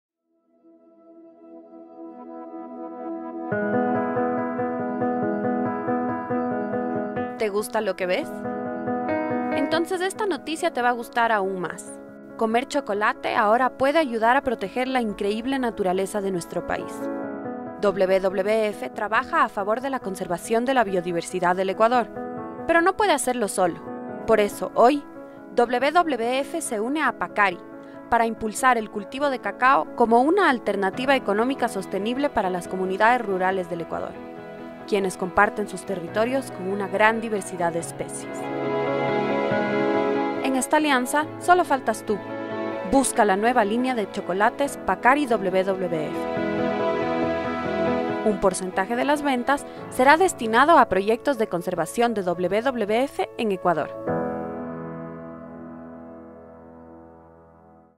Pacari Chocolate and WWF Ecuador
Pacari is working with indigenous communities in the Zancudo Concha community, in a project with WWF-Ecuador, paying and supporting them to get the organic certification, Pacari also pays 60% more than local average price providing the communities with fair and on-time payments, helping them to improve their livelihoods and protect the environment. You can also read more about this project in the WWF-Ecuador page in Spanish.